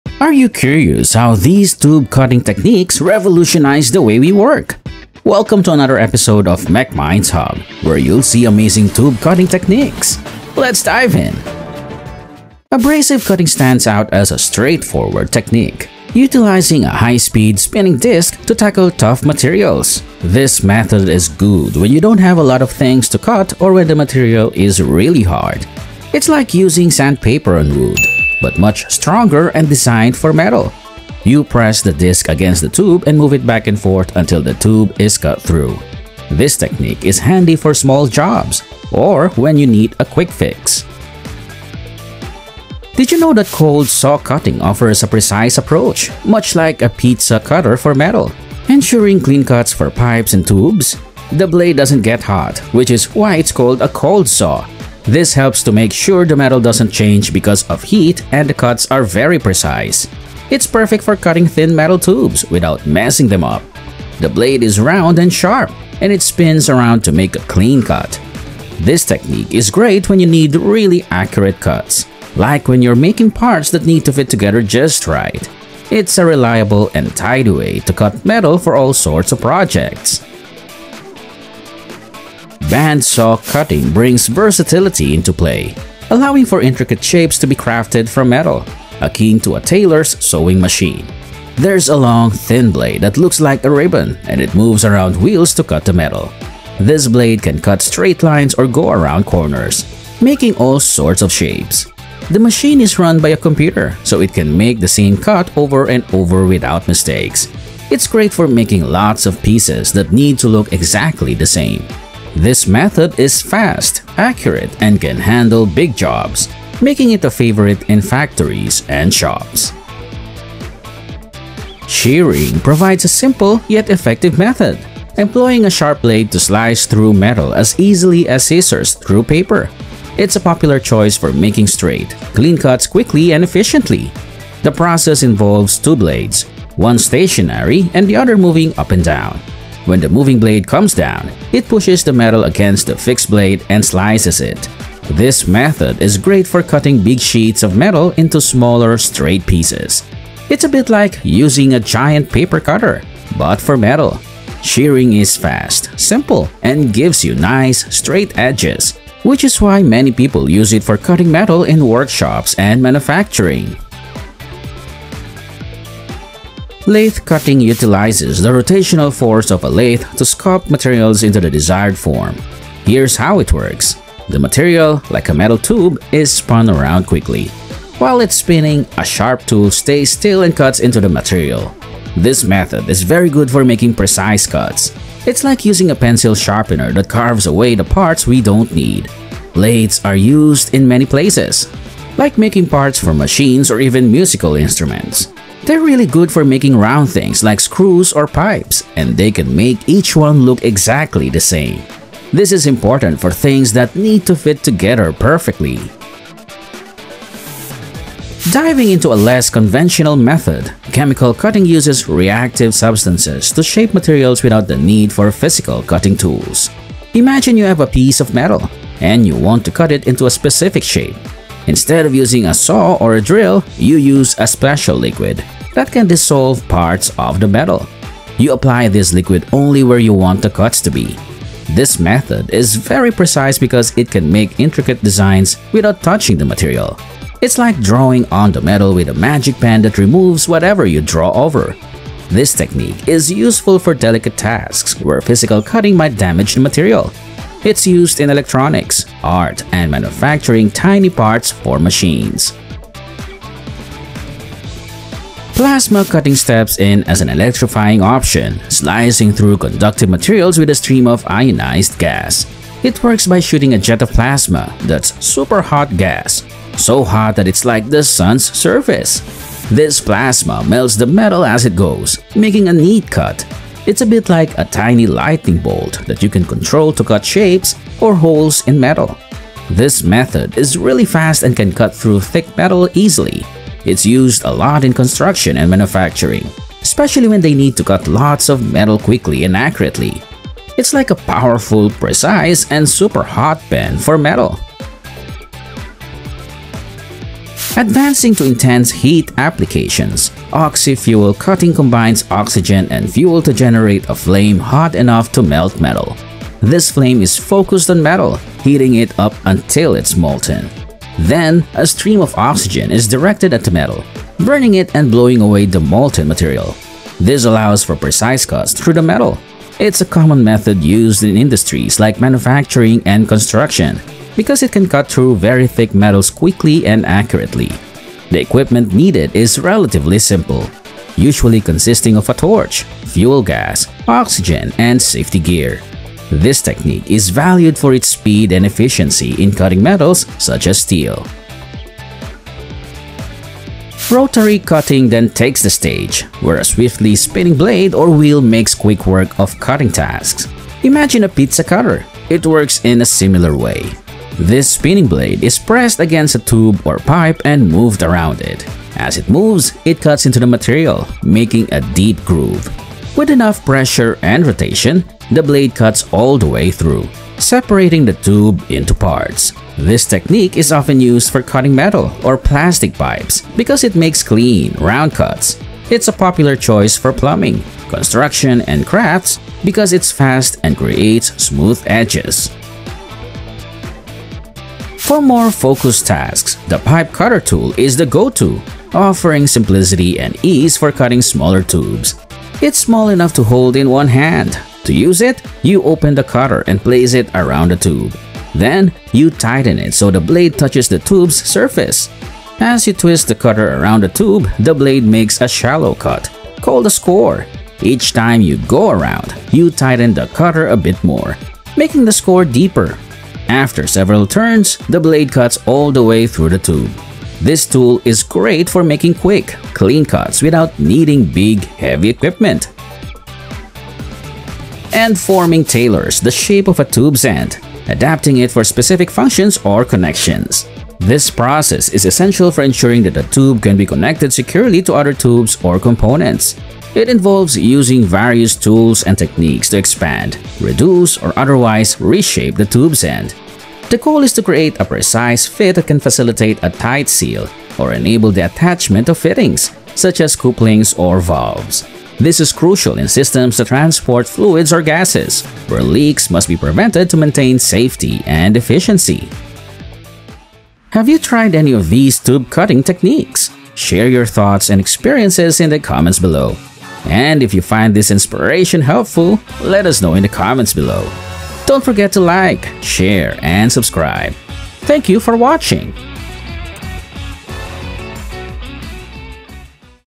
Amazing Tube Cutting Techniques You Need to See l MechMinds Hub
Learn the amazing cutting techniques that redefine precision and efficiency. Witness advanced machinery and skilled craftsmanship create flawless cuts with speed and accuracy. Whether you're a professional or just curious, these cutting-edge methods will help entertain you.